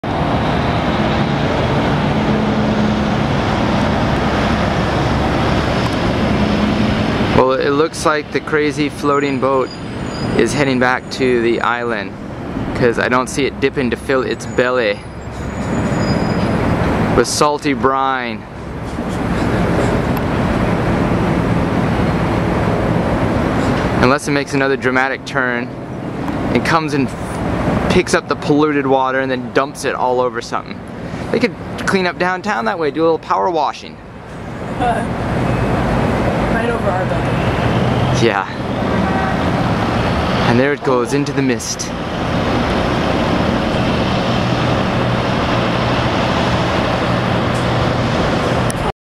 Mars Martain Flying Boat in Burrard Inlet
The Mars Martian is the largest flying boat (or something along those lines). Usually stationed on Vancouver Island, the Martian can skim the water, filling her belly with water, then gain altitude to reach the fire zone and help to douse forest fires.

In this video clip, she doesn't do the fire drill, but gives a nice slow, low pass over Vancouver's Burrard Inlet  while my pal Richard and I observe from the top floor of Gastown  building.